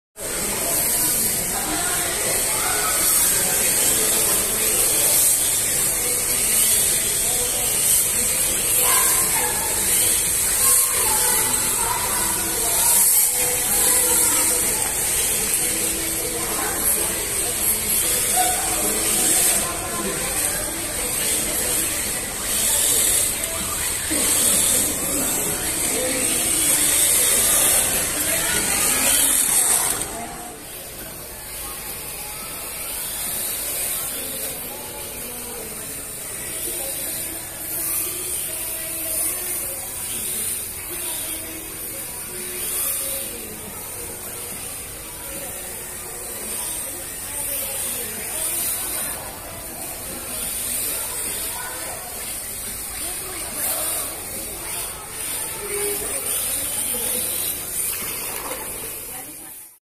Rastar 1/10 Drift Mod vs Real RC Drift feat @RaizamalikChannel as Rastar Drifter
Tes RC QD Rastar yang telah di mod menjadi RC Drift Propro di Blok M Mall.
RC Drift murah ala-ala vs RC Drift mahal, masih pake brushed motor.